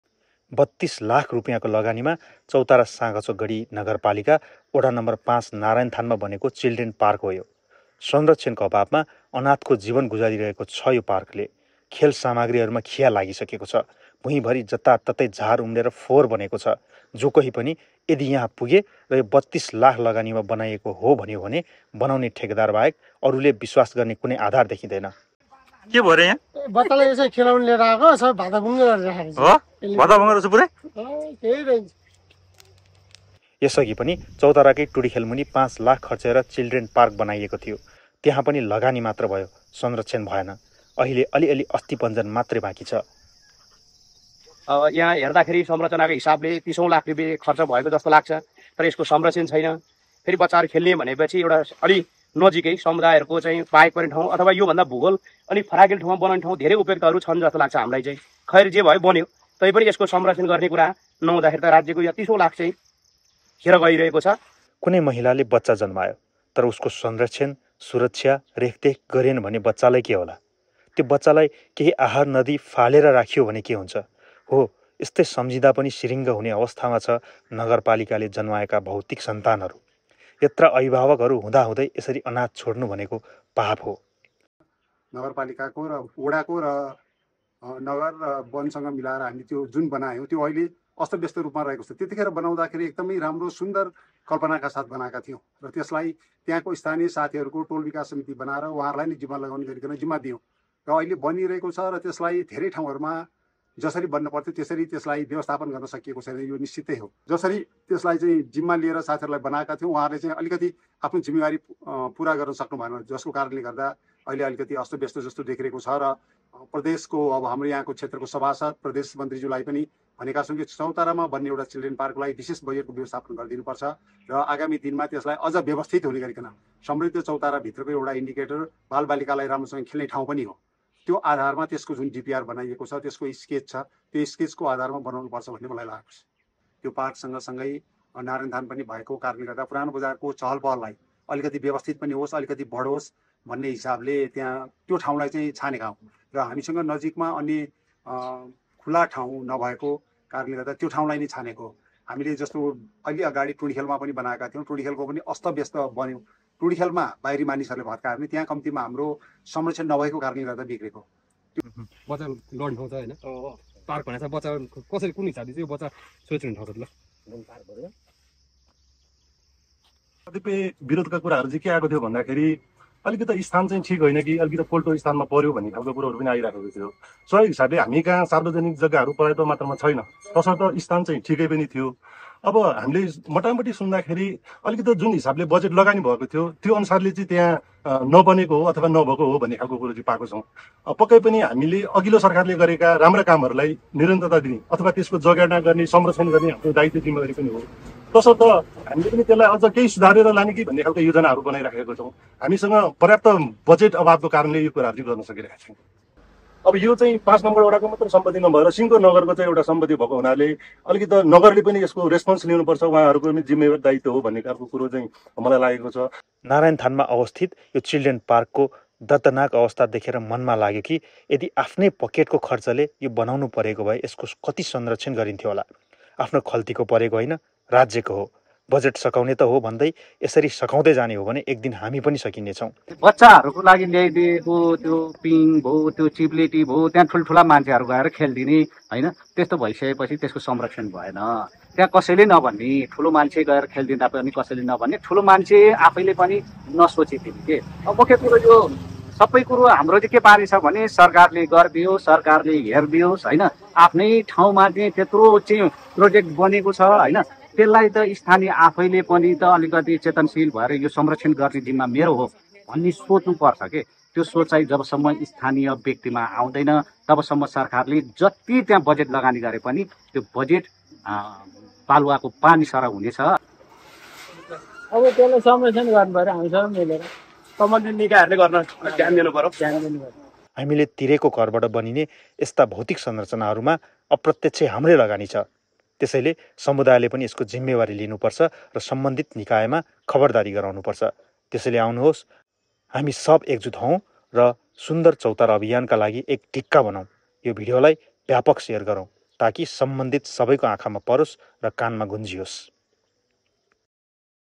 राज्यको लगानी कसरी खेर गइरहेको छ ? चिल्ड्रेन पार्कको अवस्था । जिम्मेवारी कसको ?
Children Park of Chautara. राज्यको लगानी कसरी खेर गइरहेको छ ?